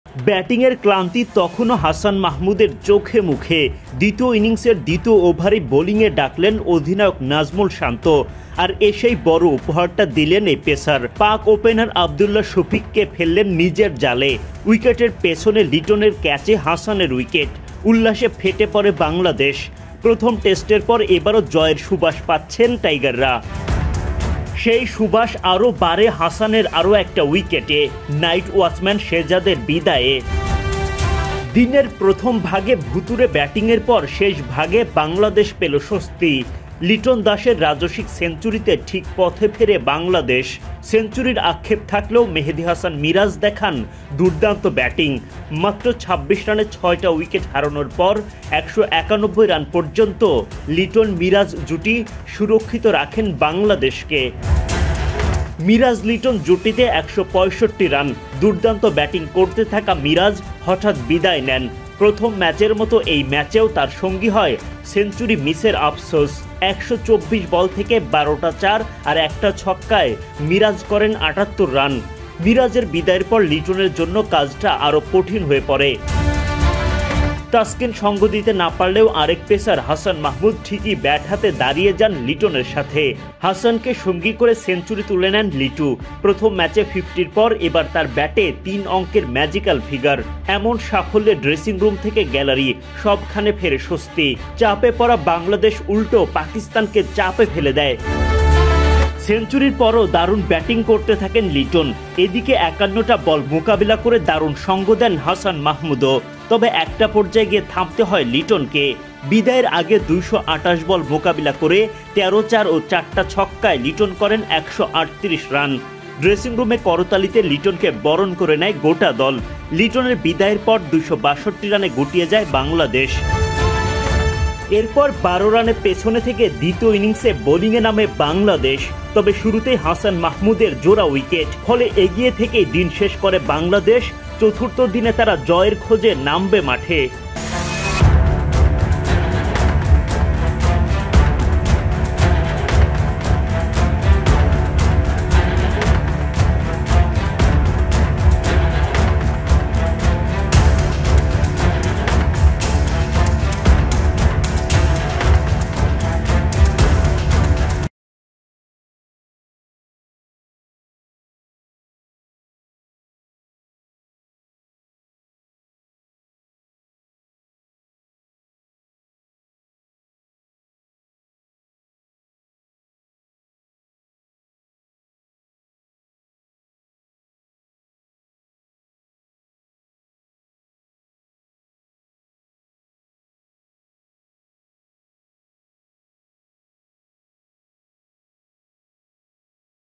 হাসানের ১০ বলে ২ উইকেট, লিটন-মিরাজের ব্যাটে কামব্যাক; জয়ের সুবাস পাচ্ছে বাংলাদেশ | Hasan | Litton
ব্যাটিংয়ের ক্লান্তি তখনও হাসান মাহমুদের চোখে-মুখে। দ্বিতীয় ইনিংসের দ্বিতীয় ওভারেই বোলিং ডাকলেন অধিনায়ক নাজমুল শান্ত। আর এসেই বড় উপহারটা দিলেন এই পেসার। পাক ওপেনার আবদুল্লাহ শফিককে ফেললেন নিজের জালে। উইকেটের পেছনে লিটনের ক্যাচে হাসানের উইকেট। উল্লাসে ফেটে পড়ে বাংলাদেশ। প্রথম টেস্টের এবারও জয়ের সুবাস পাচ্ছেন টাইগাররা। *সেই সুবাস আরও বাড়ে নাইটওয়াচম্যান শেহজাদের বিদায়ে।